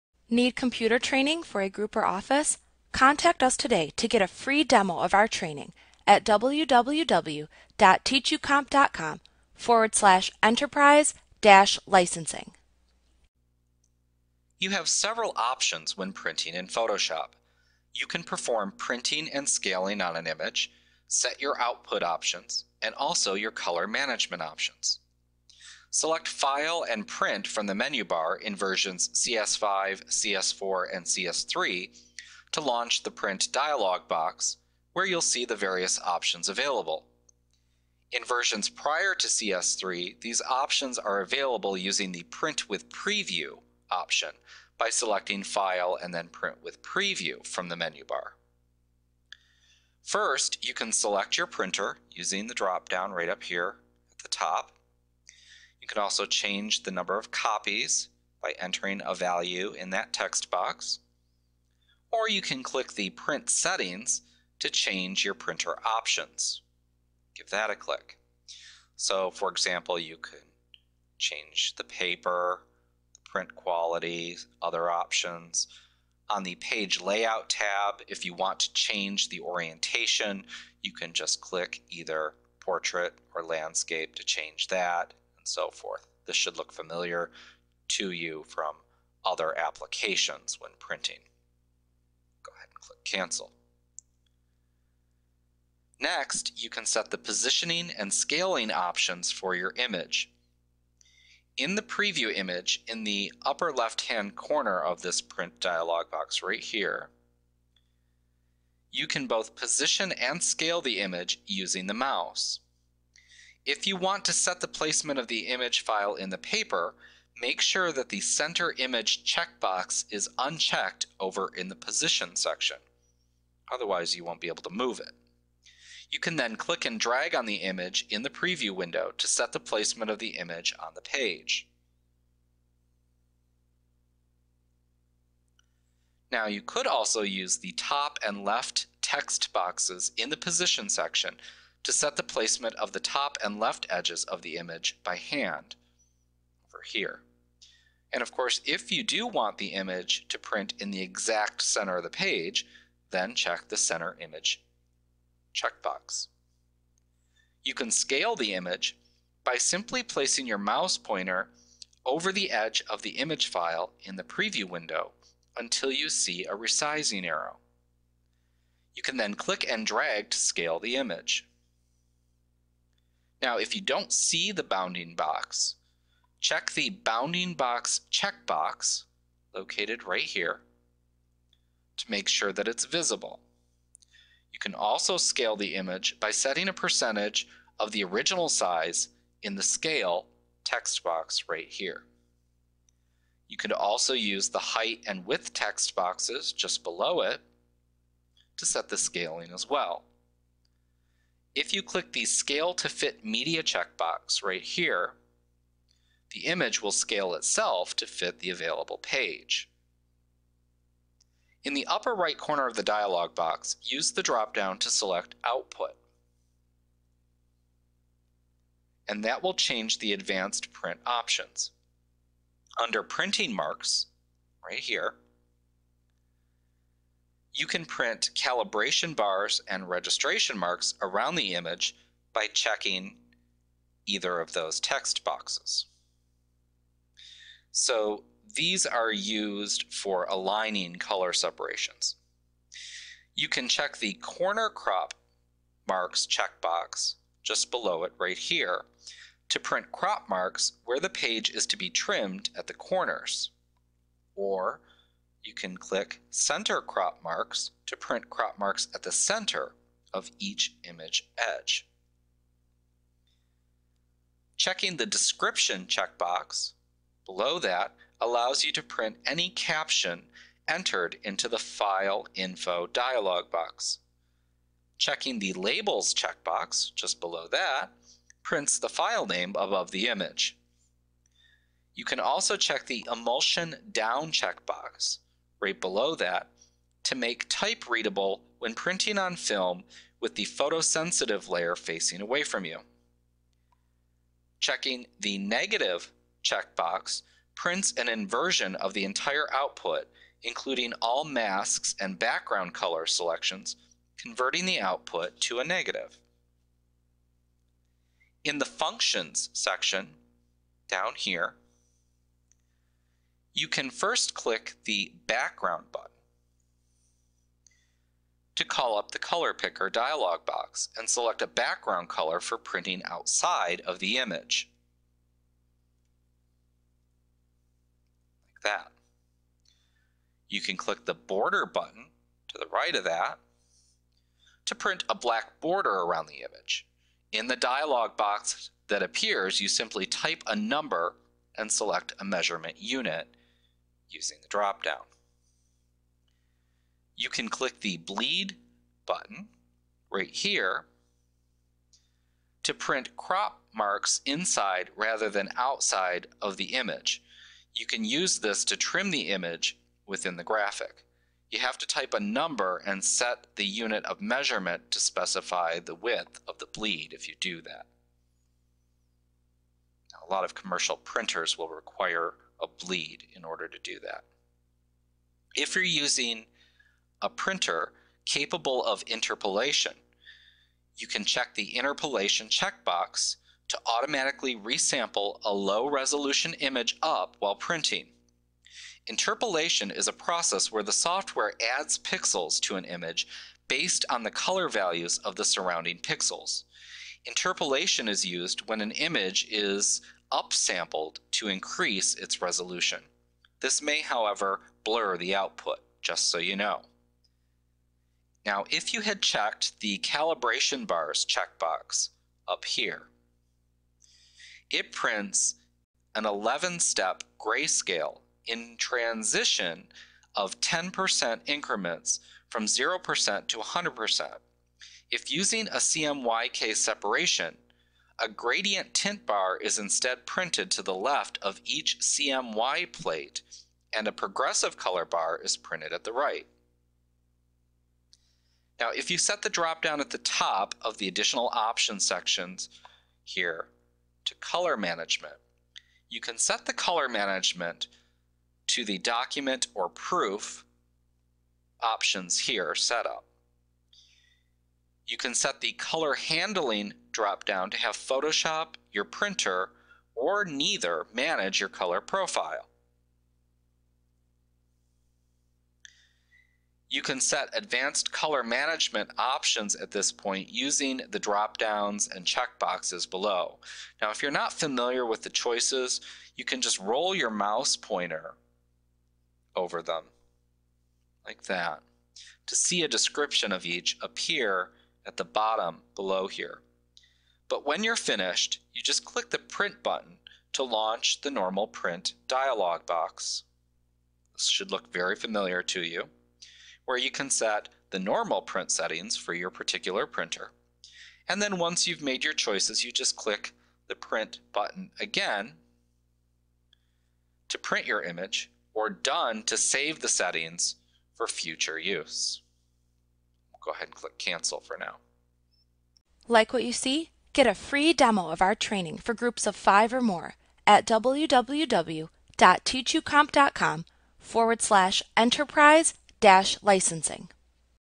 Photoshop CS6 Tutorial Setting Printing Options Adobe Training
FREE Course! Learn how to set printing options in Adobe Photoshop A clip from Mastering Photoshop Made Easy v. CS6. - the most comprehensive Photoshop tutorial available. Visit us today!